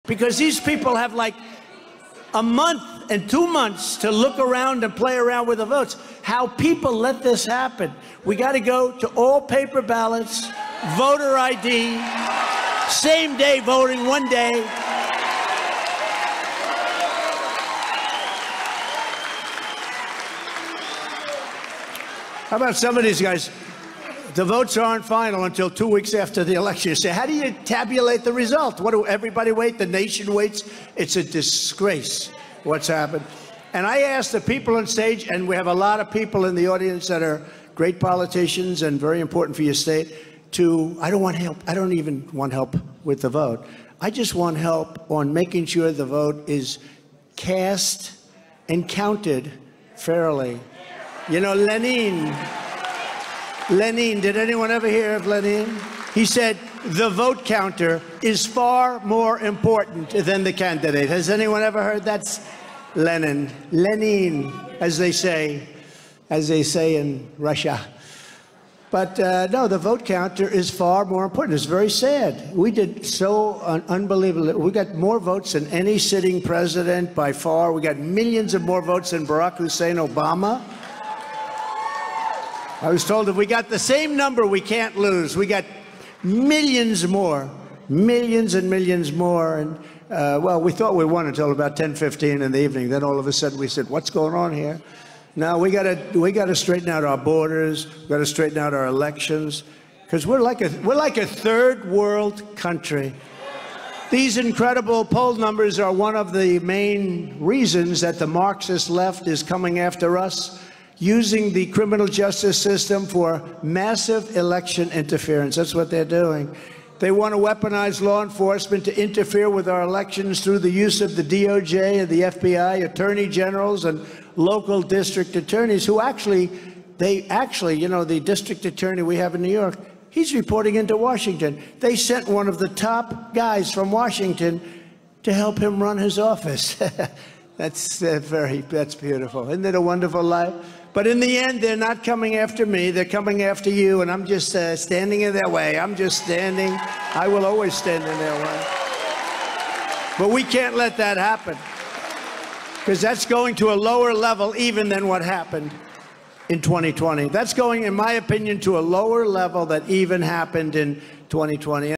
Trump 2024 | Counting the Votes
President Donald J. Trump speaking in New Hampshire, April 27, 2023.